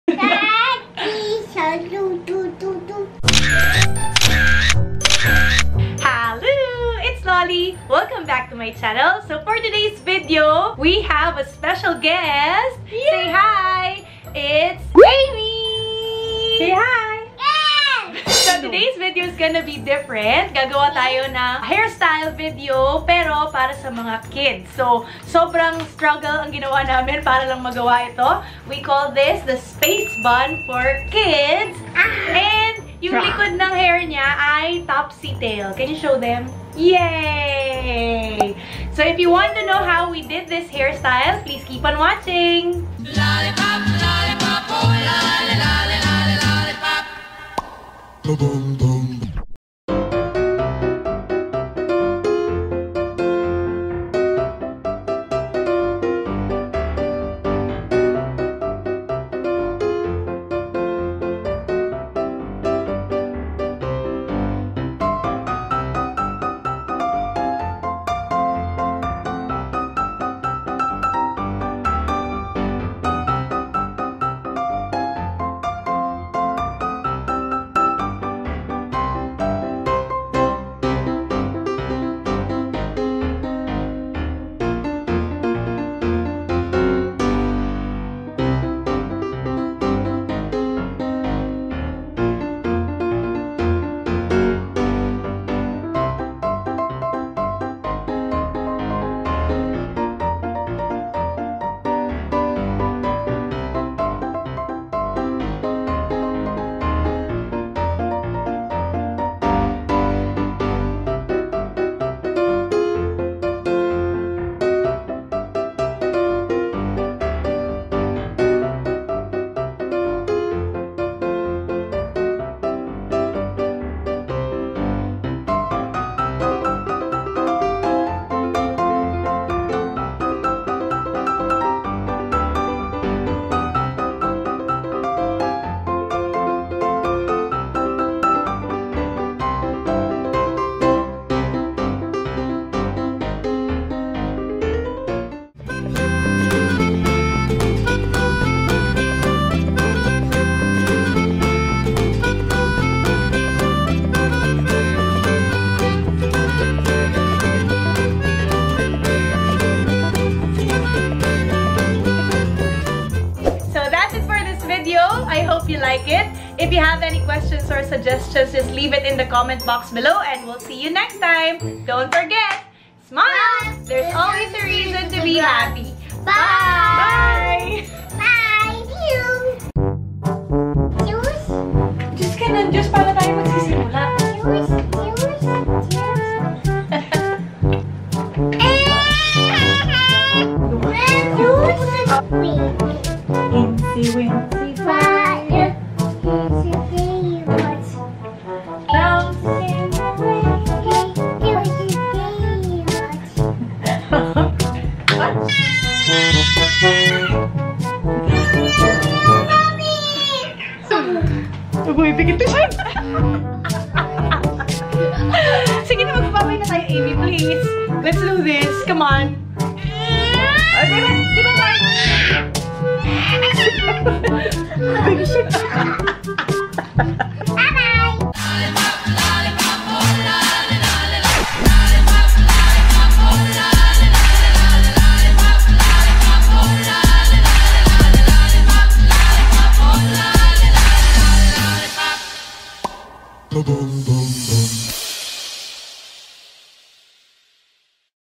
HOW TO: HAIRSTYLE FOR BABIES (SHE GOT MAD!) | SPACE BUN WITH TOPSY TAIL| Lolly Isabel
In this video, I created a space bun with upside down topsy tail for my cute little niece, Amy. It's a struggle to style babies' hair since they keep on moving around and they don''t want other people touching their hair. BUT...WE DID IT!

With the help of a lot of distractions, we were able to make this cute hairstyle for babies :) ENJOY!

💋 Lolly